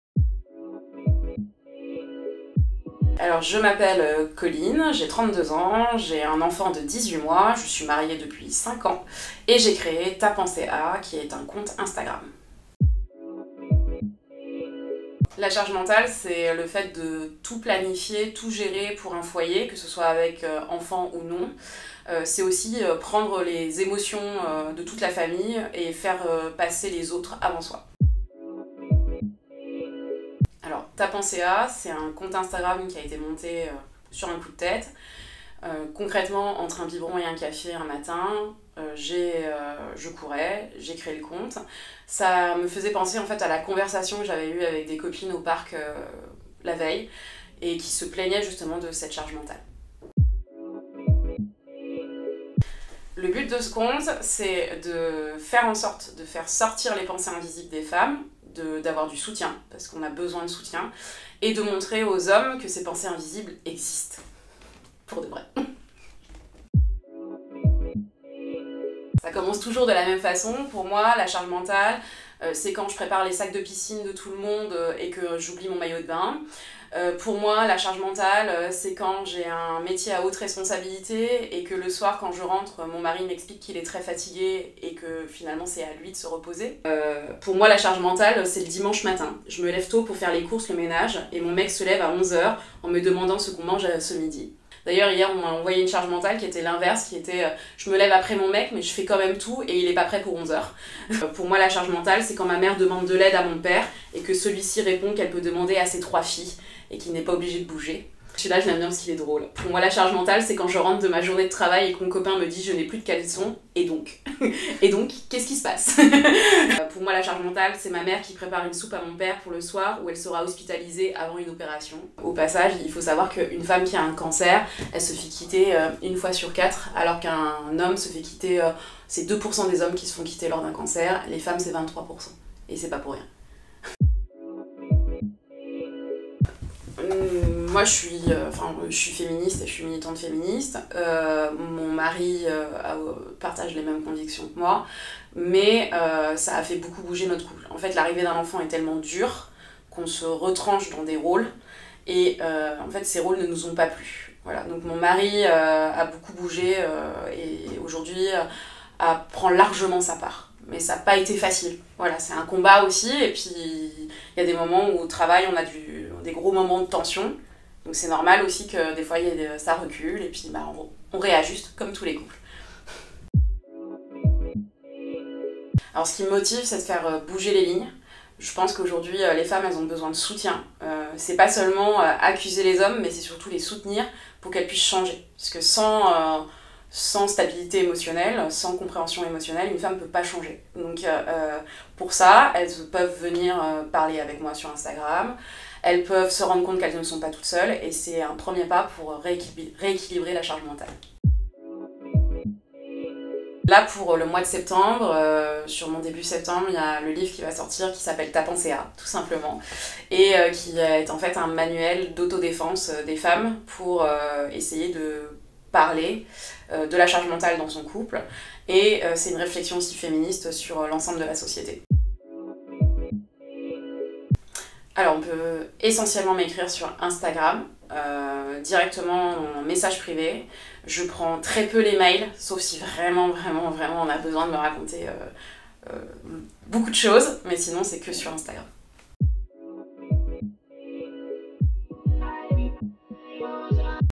[INTERVIEW] Coline, de T'as pensé à ?
On a eu le plaisir de rencontrer Coline, créatrice du compte T'as pensé à ? pour nous parler de la Charge Mentale ! Si vous ne connaissez pas ➡️